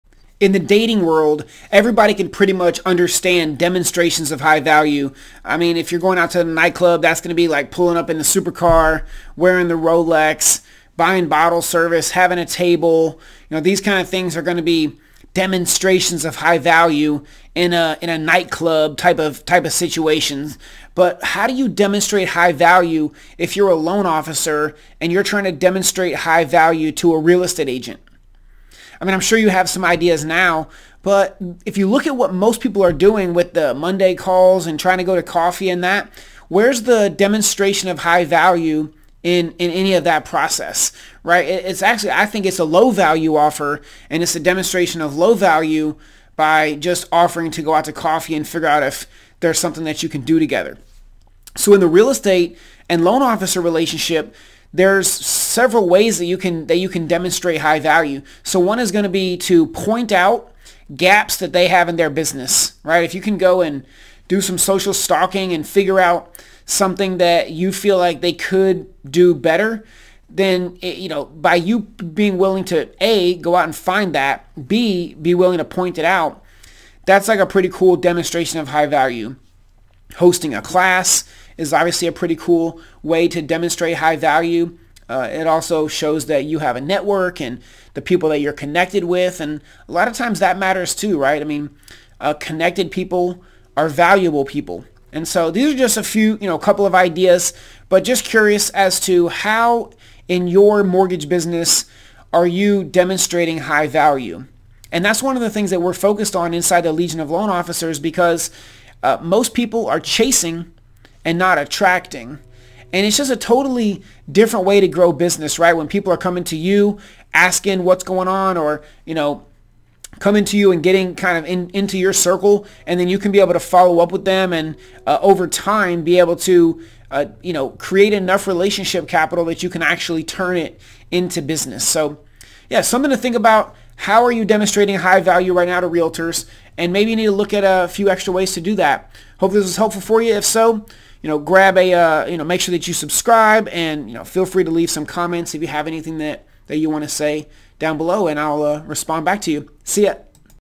Loan Officer Demonstrations of High Value for Realtors
Most guys understand demonstrating high value in the dating game but aren't sure how to do it in the Loan Officer - Realtor relationship. Nick Carpenter shares a few ways to use this in your mortgage business. Need help? Apply tojoin The Legion of Loan Officers

Take the 6 Week LO Challenge and add 3 Realtor partners in 42 days